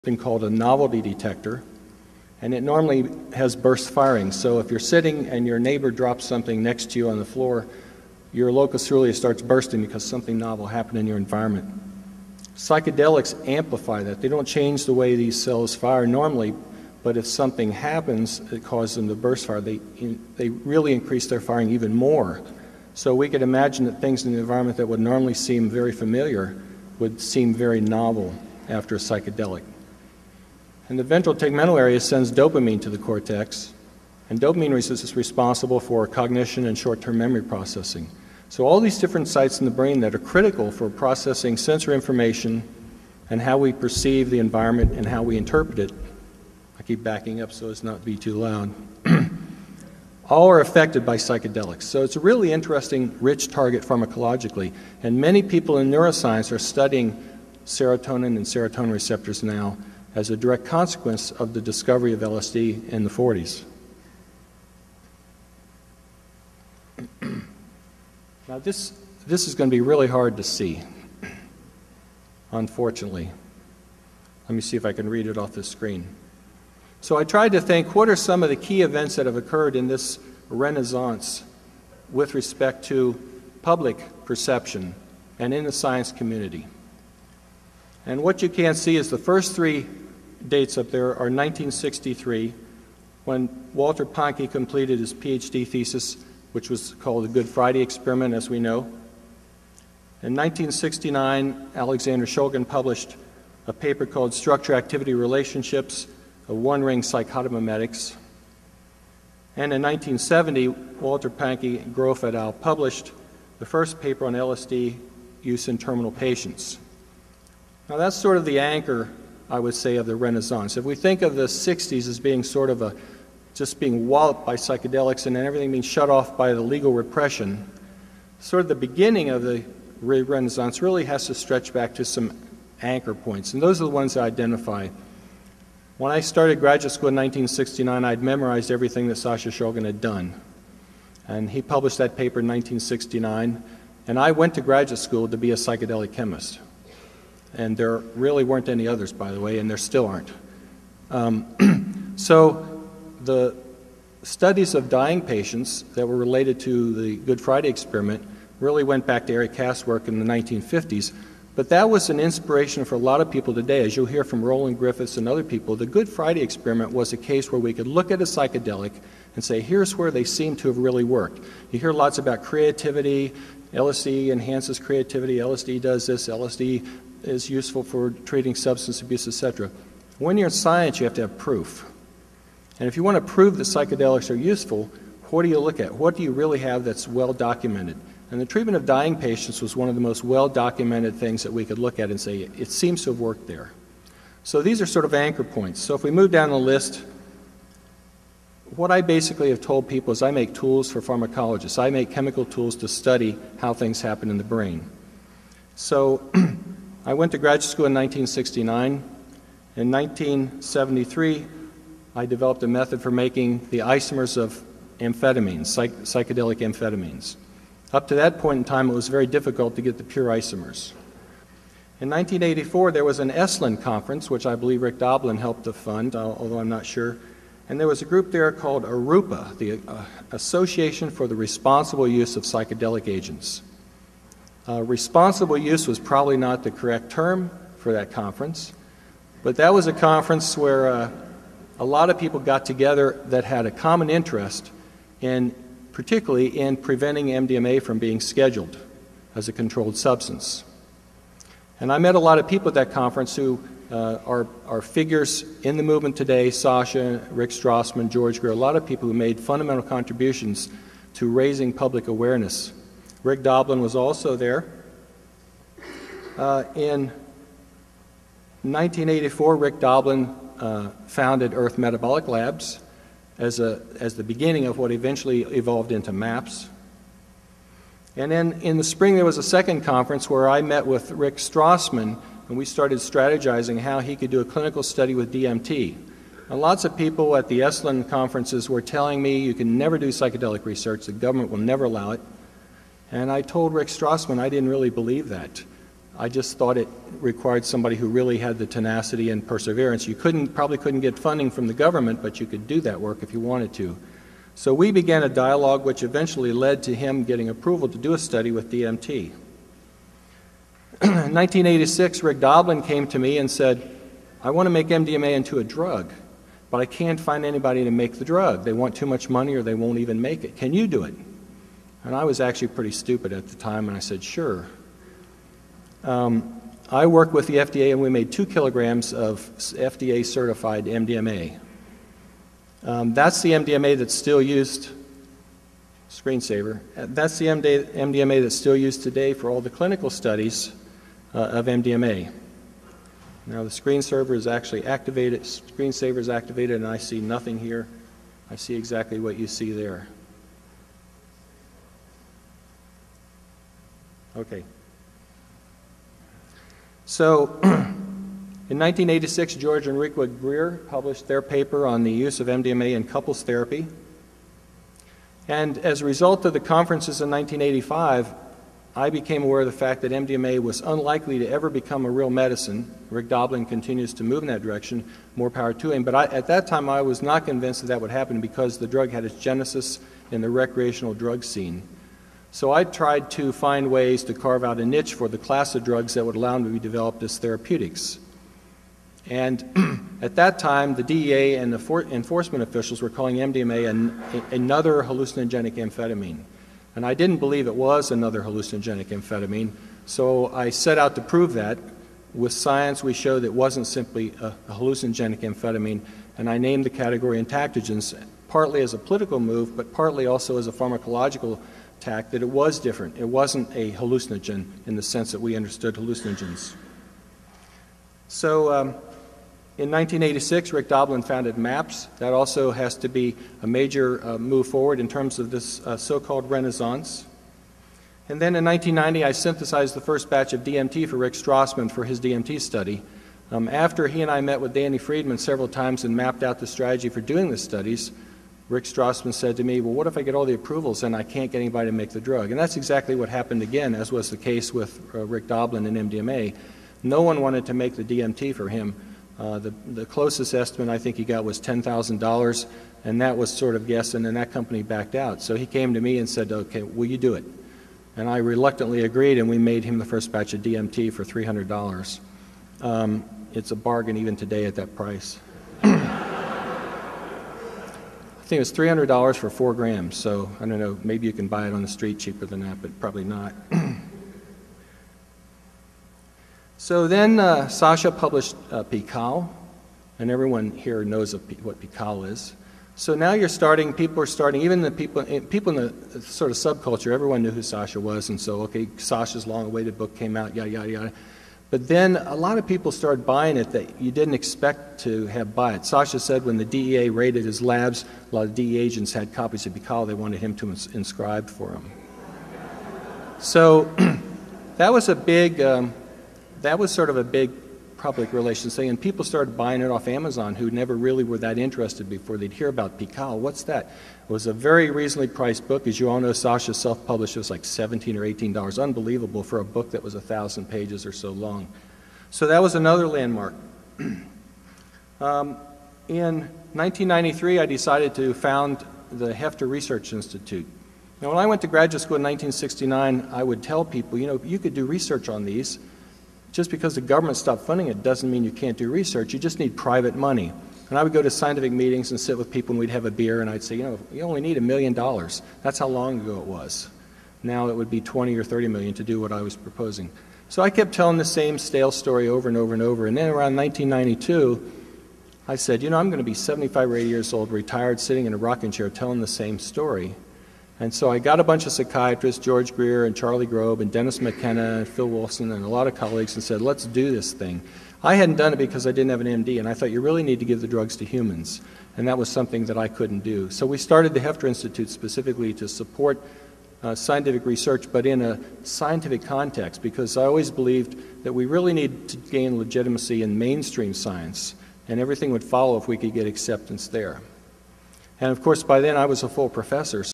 David E. Nichols @ Horizons'08 (2/5)
Is There a Renaissance in Psychedelic Research and How did it Happen?

The definition of renaissance is: a renewal of life, vigor, interest, etc.; rebirth; revival. If there is a renaissance in psychedelic research, to what or to whom can it be attributed? Did it just happen spontaneously? This talk will provide an overview of several events that may have contributed to the genesis of this revival. Incorporation of the Heffter Research Institute in 1993 was one such event, but many individuals played key roles in a process that has taken place over at least three decades and has led us to the present moment. This renaissance, if we can call it that, has resulted from the dedication and work of a relatively few individuals who believed strongly that psychedelics did not get a fair shake in their earlier incarnation in Western society and who have worked to educate the public and to increase understanding of the actual nature of these substances.

Biography

David E. Nichols, Ph.D. is a Professor of Medicinal Chemistry and Pharmacology in the School of Pharmacy and Pharmaceutical Sciences at Purdue University, as well as a co-founder of the Heffter Institute, where he serves as Director of Preclinical Research.

Dr. Nichols has published more than 200 research articles on various aspects of the medicinal chemistry and neuropharmacology His laboratory has published numerous studies elucidating details both of the mechanism of action of MDMA and of the biochemical events related to the neurotoxic effects seen in animals following administration. Dr. Nichols coined the name 'entactogen' to describe the unique psychopharmacological effects of MDMA and related compounds.